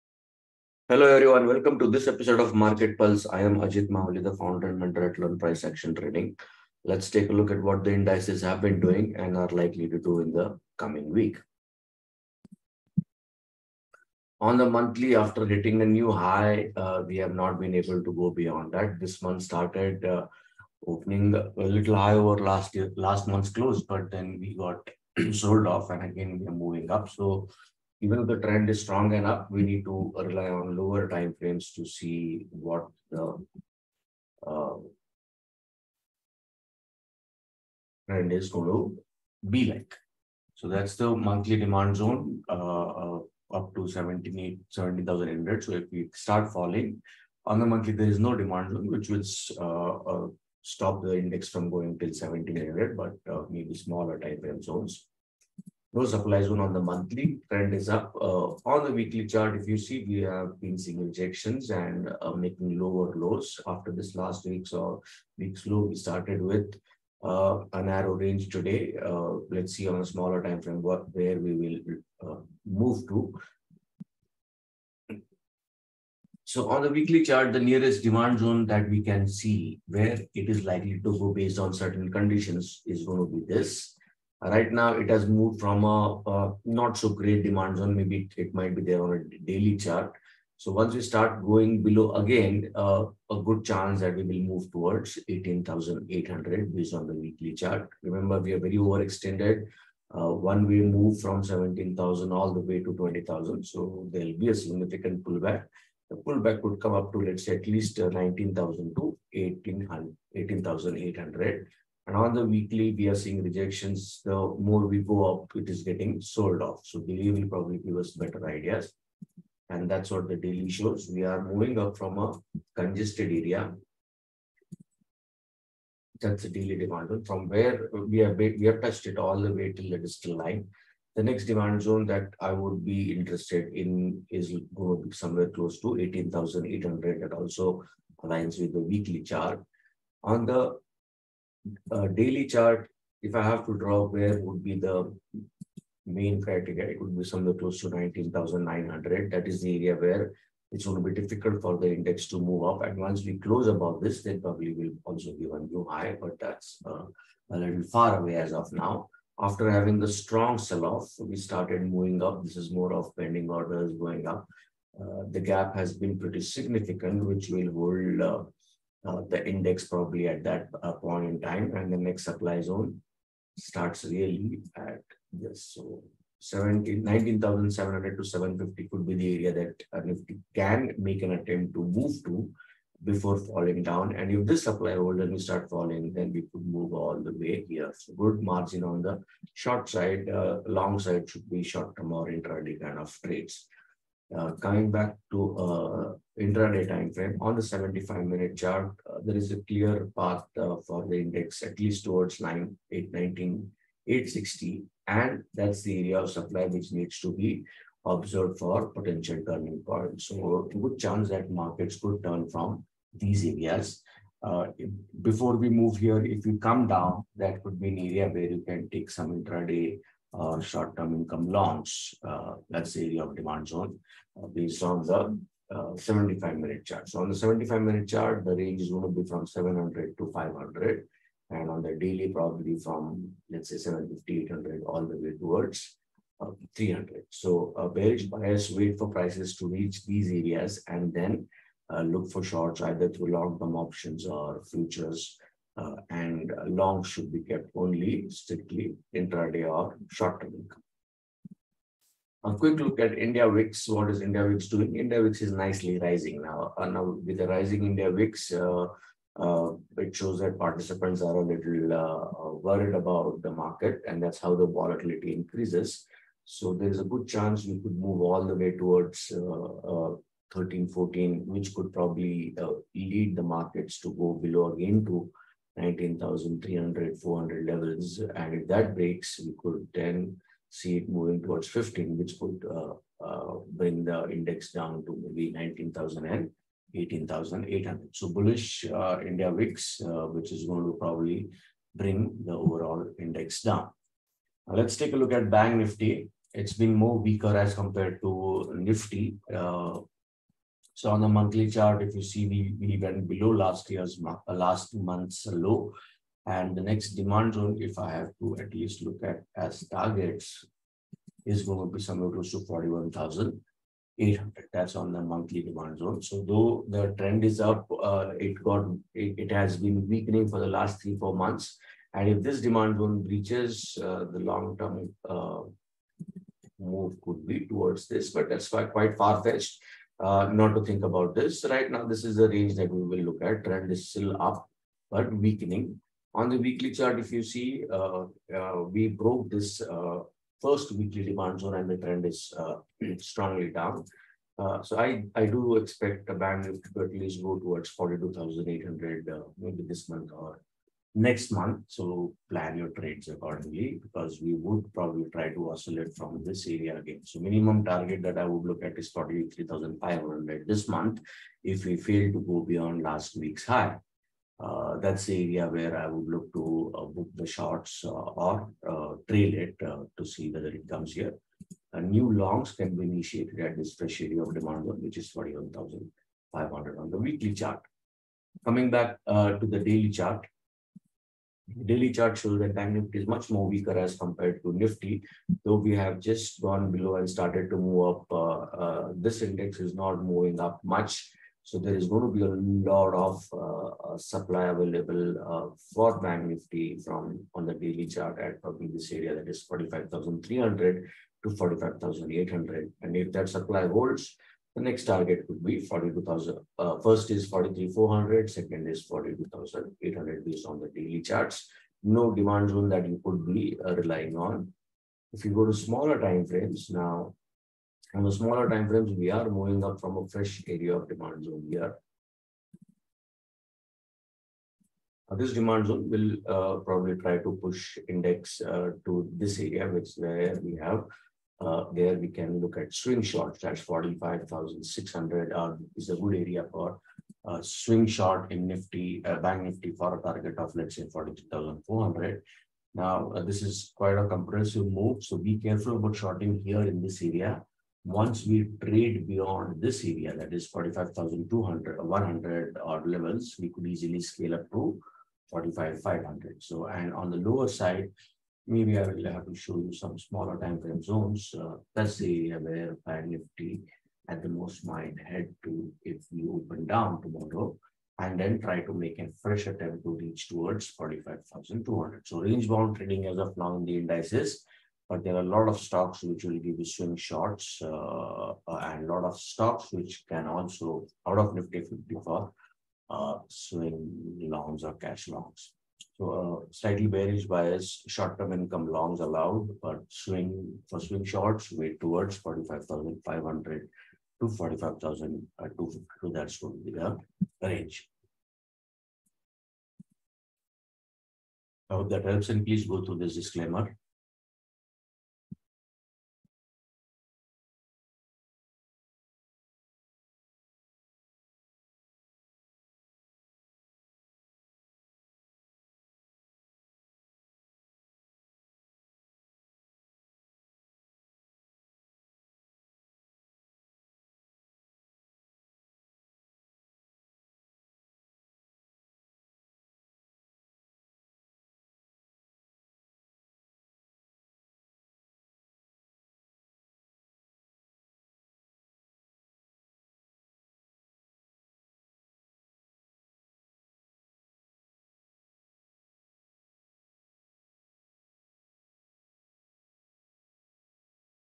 Weekly Index Analysis: What to Expect from Nifty & Bank Nifty This Week (7th Aug - 13th Aug 2023)
📺 Check out Market Pulse with LPAT Founder and Mentor Ajit Mahuli! Discover Nifty and Bank Nifty Indices' potential movements for the week of 7th August 2023 to 13th August 2023. 🔍 Get valuable index analysis insights that could shape your trading/investment decisions!